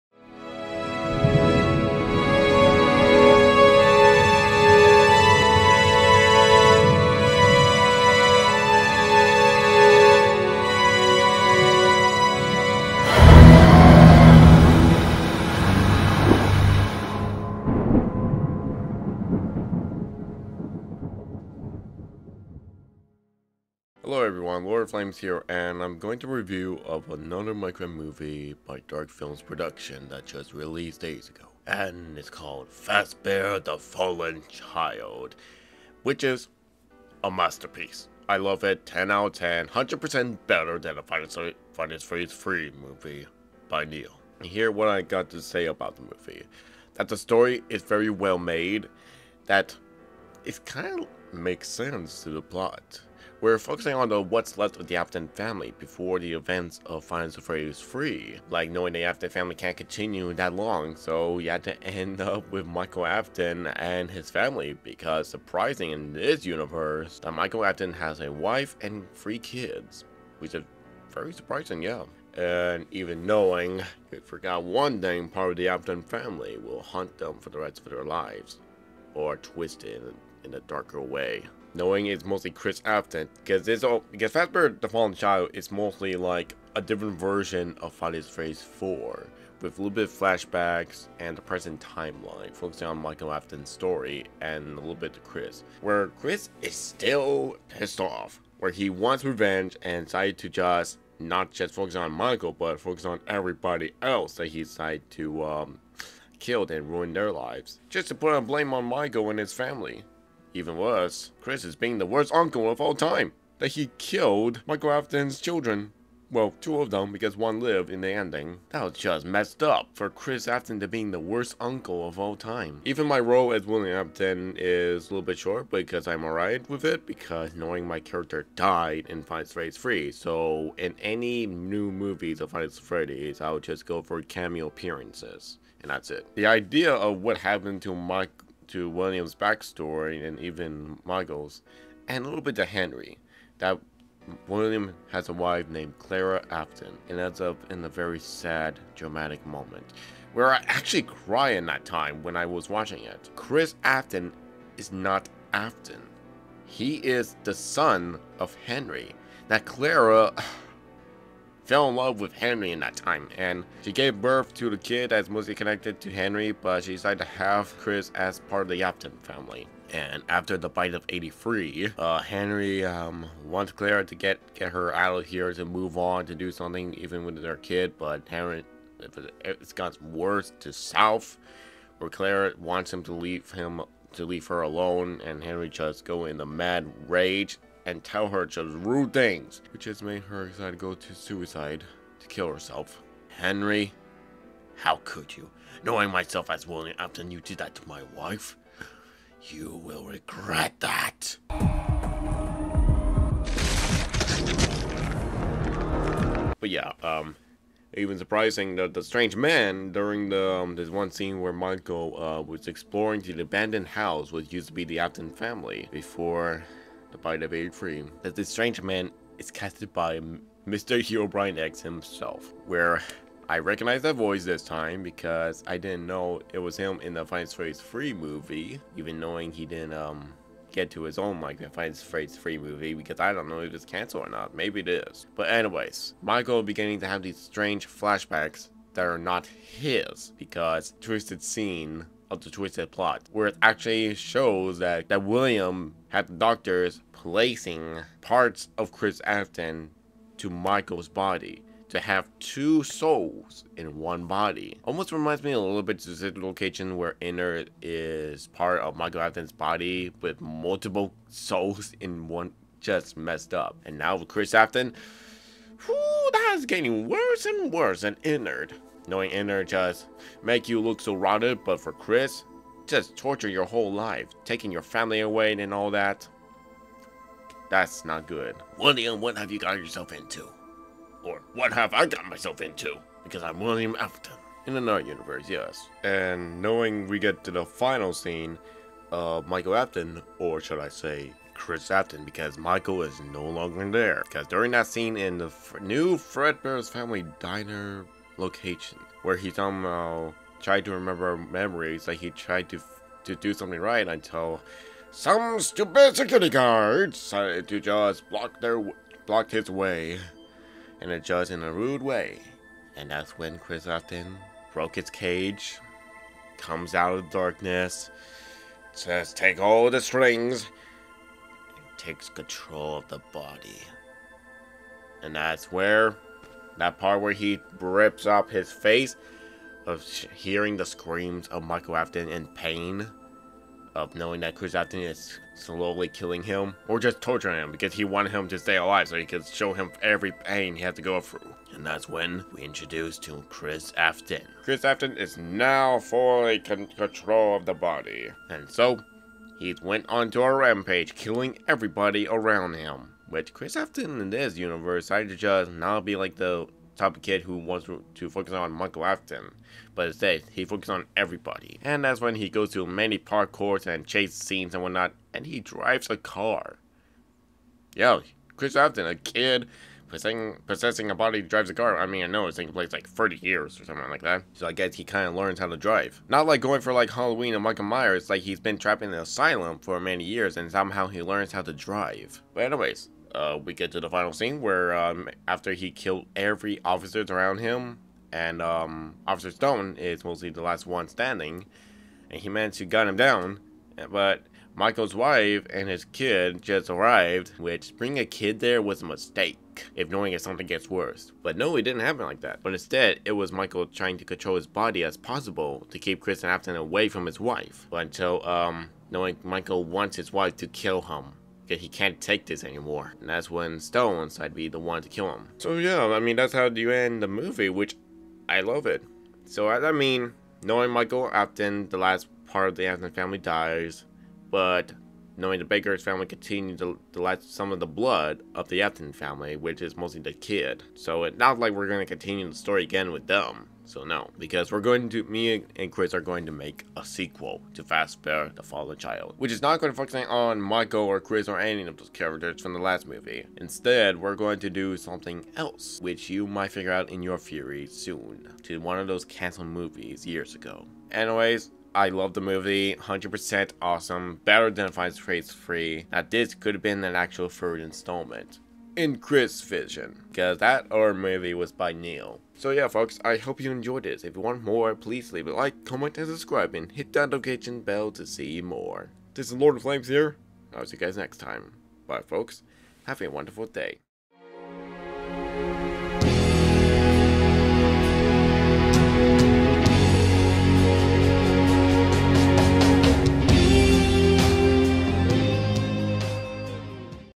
My THOUGHTS on Fazbear: The Fallen Child
Here we are on another review video, I decided to have this reaction video as a review because I got an error on my recording footage after I finished watching the movie. So today I'll review and have my thoughts on the movie that Darkfilmz Productions released. Fazbear: The Fallen Child! Hope you enjoy this.